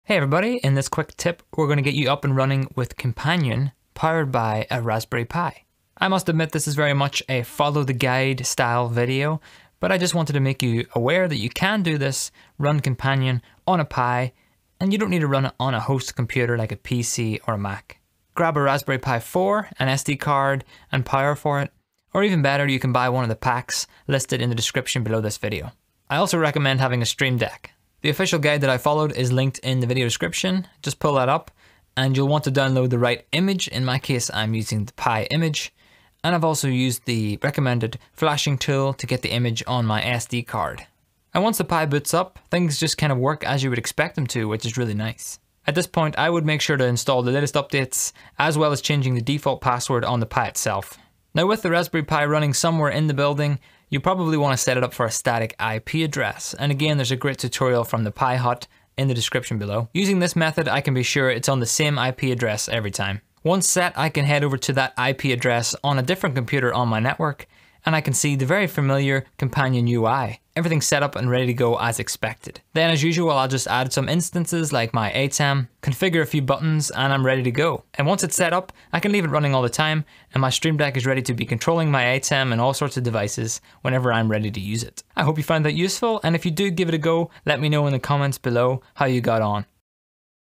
Run Companion on a Raspberry Pi // Quick Tip
Getting Bitfocus Companion running on Pi is more simple than you might think.

*Timestamps*
00:00 - Intro
00:18 - What you'll need
00:28 - The guide
00:51 - Static IP address
01:03 - Up and running!

Raspberry Pi kit